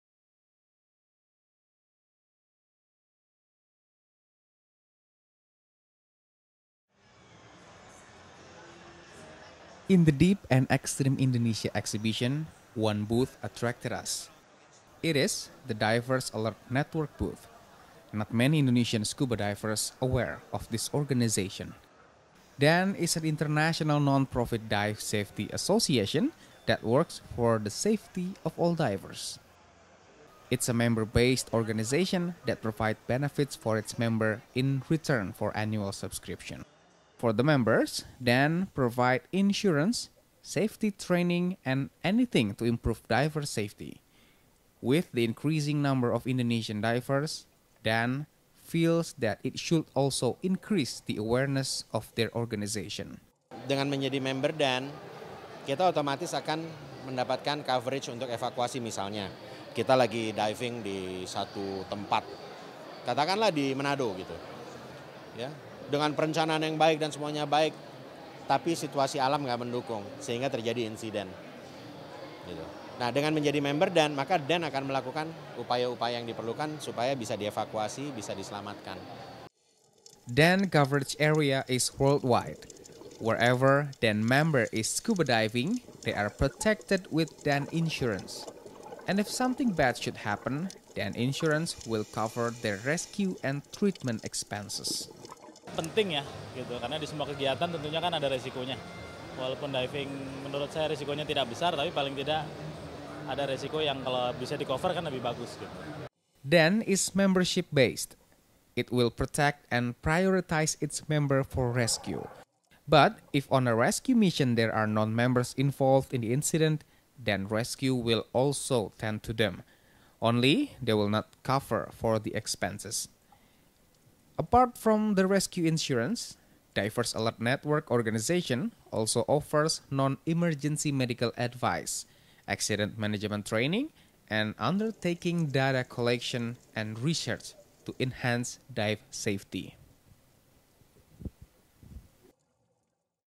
Divers Alert Network insurance
Divers Alert Network or DAN is a member based non-ptofit organization that provide many kind of scuba diving safety measures. DAN insurance specialized in scuba diving, offering protection and assurance in rescue and scuba diving related injury.